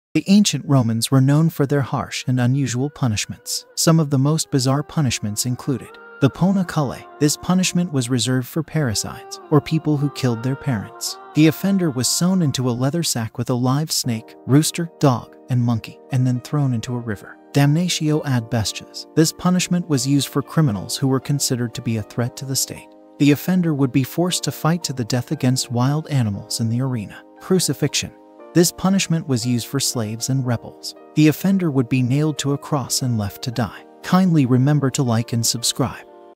Bizarre Punishments from Ancient Rome
The ancient Romans were known for their harsh and unusual punishments. In this video, we will explore some of the most bizarre punishments that were used in ancient Rome, including the poena cullei, damnatio ad bestias, and crucifixion.

We will discuss the history of these punishments, how they were used, and why they were eventually outlawed. We will also explore the legacy of these punishments and their impact on modern society.

This video is a reminder of the dark side of human history, but it is also a reminder of the importance of fighting for justice and compassion.

What do you think of the bizarre punishments of ancient Rome? Do you think they were justified?